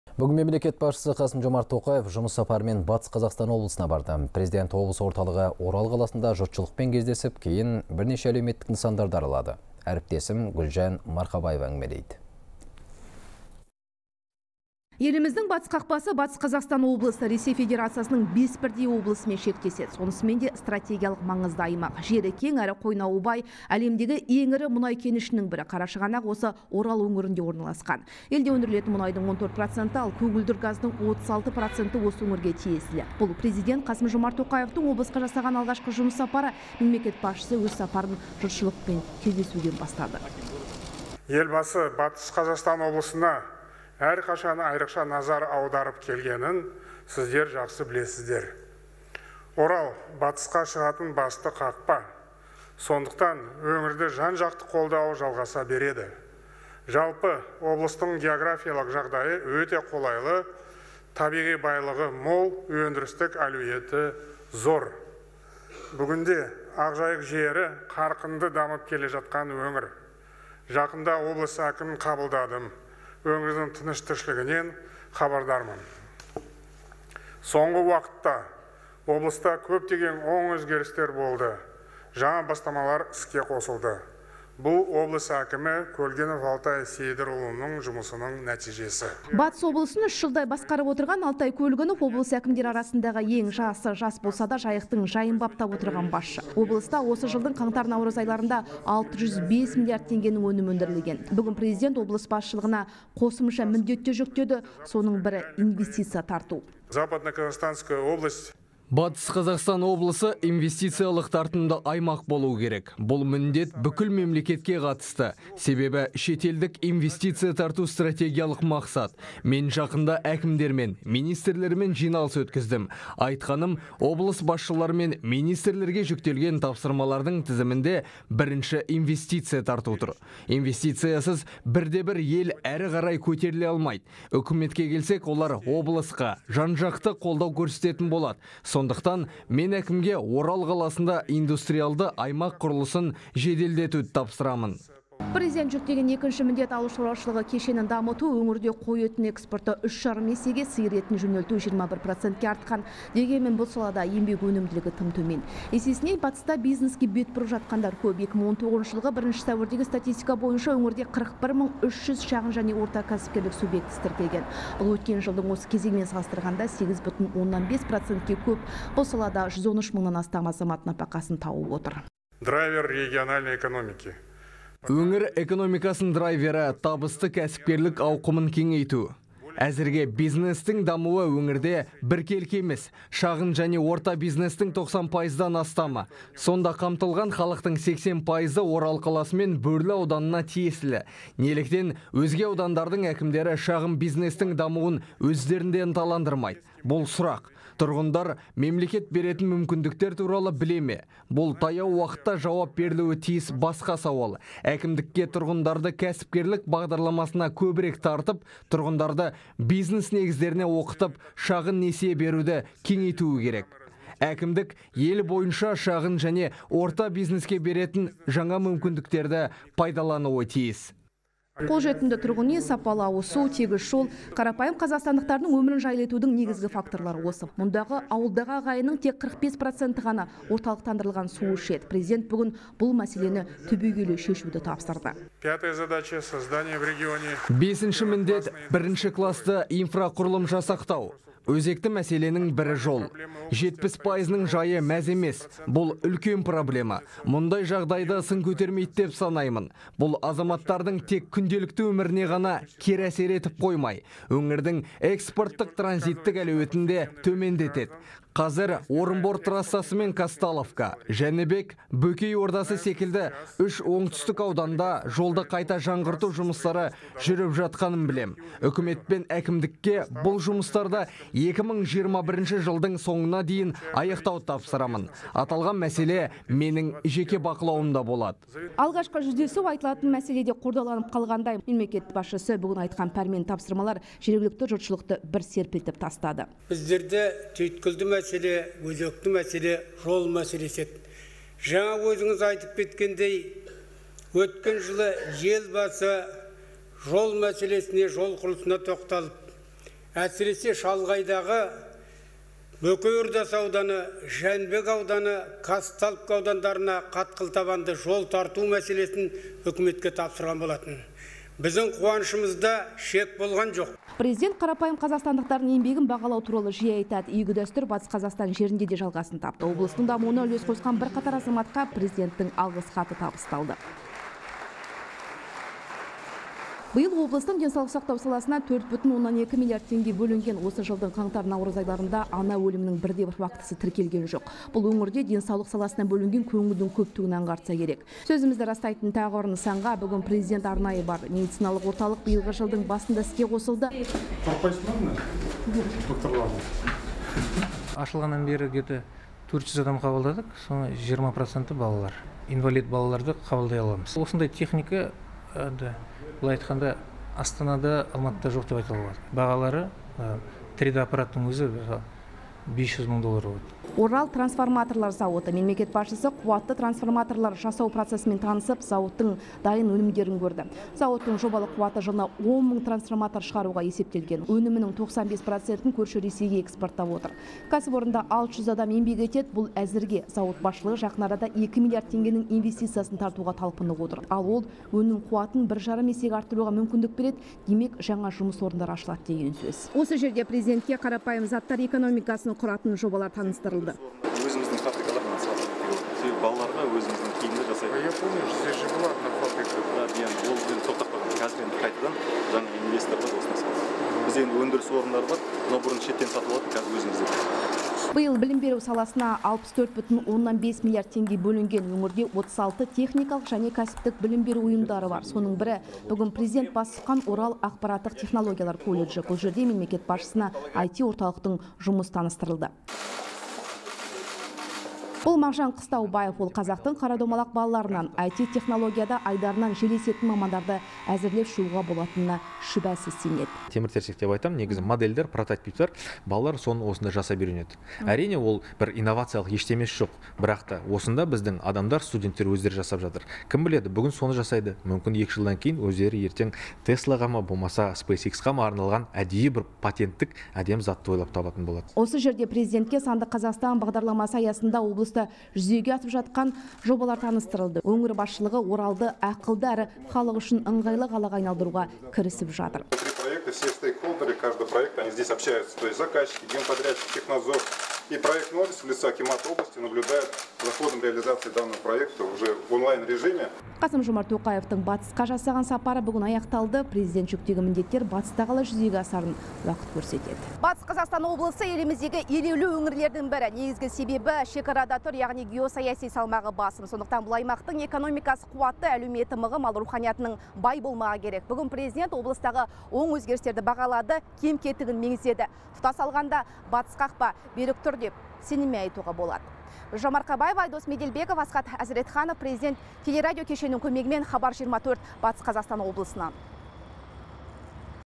Қасым-Жомарт Тоқаев: Батыс Қазақстан облысы инвестициялық тартымды аймақ болуы керек
Бүгін Мемлекет басшысы Қасым-Жомарт Тоқаев жұмыс сапарымен Батыс Қазақстан облысына барды, деп хабарлайды «Хабар 24».

Президент облыс орталығы Орал қаласында жұртшылықпен кездесіп, кейін бірнеше әлеуметтік нысанды аралады.

Еліміздің батыс қақпасы – Батыс Қазақстан облысы. Ресей Федерациясының 5 бірдей облысымен шектеседі. Сонысымен де стратегиялық маңызды аймақ. Жері кең әрі қойнауы бай. Әлемдегі ең ірі мұнай кенішінің бірі Қарашығанақ осы Орал өңірінде орналасқан. Елде өндірілетін мұнайдың 14%, ал көгілдір газдың 36% осы өңірге тиесілі. Бұл – Президент Қасым-Жомарт Тоқаевтың облысқа жасаған алғашқы жұмыс сапары. Мемлекет басшысы өз сапарын жұртшылықпен кездесуден бастады.